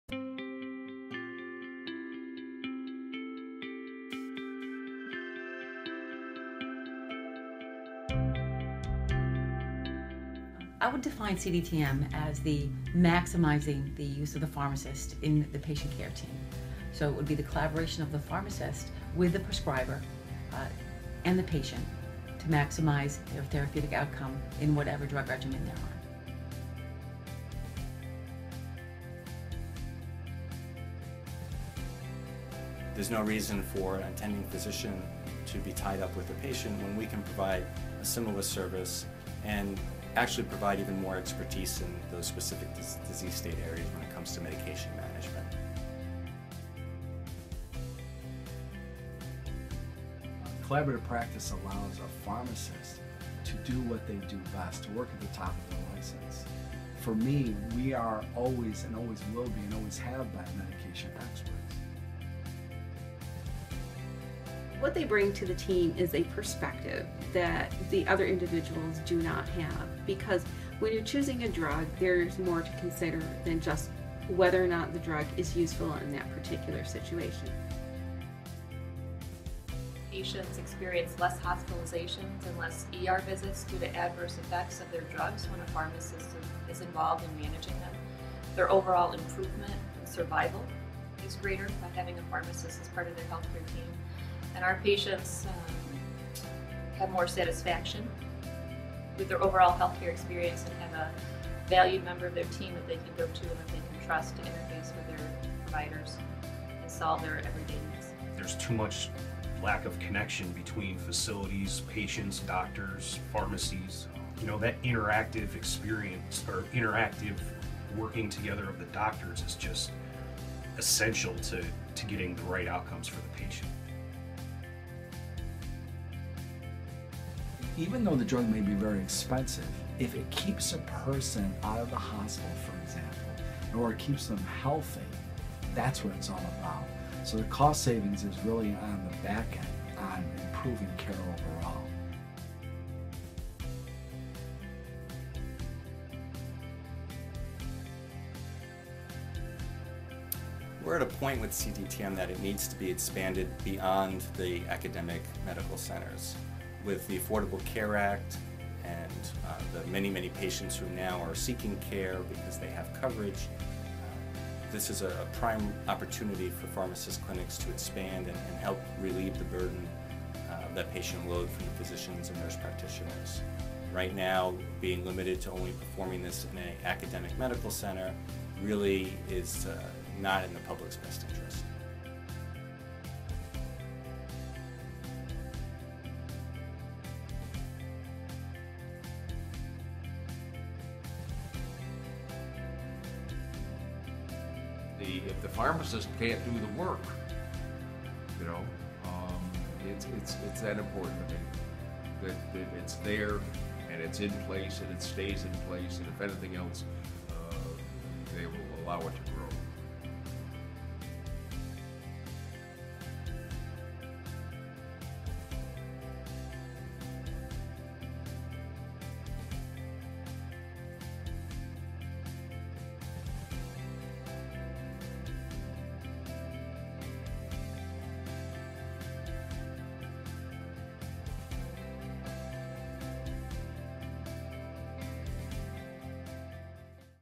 Collaborative Drug Therapy Management in New York State
EDITORS NOTE: The PharmD is a 4 year professional program and while most of the ACPHS students have 6 years of education, nationwide, 80% of students have 3 years of pre-pharmacy coursework and 55% hold BS degrees.

Produced by: Student College of Clinical Pharmacy at Albany College of Pharmacy and Health Sciences

Advisors: Dr. Amy Barton Pai & Dr. Kate Cabral

Contributors (in order of appearance): Dr. Leigh Briscoe-Dwyer, Dr. Robert DiGregorio, Dr. Joe Anderson, Dr. Mark Sinnett, Dr. Kimberly Zammit, Dr. Kelly Rudd, Eric Shlapack & Douglas Hamilton

Funding by: SCCP-ACPHS

For more information on the CDTM legislation please visit the PSSNY

If you are a NYS constituent, please contact your legislators to support this law and send them a link to this video!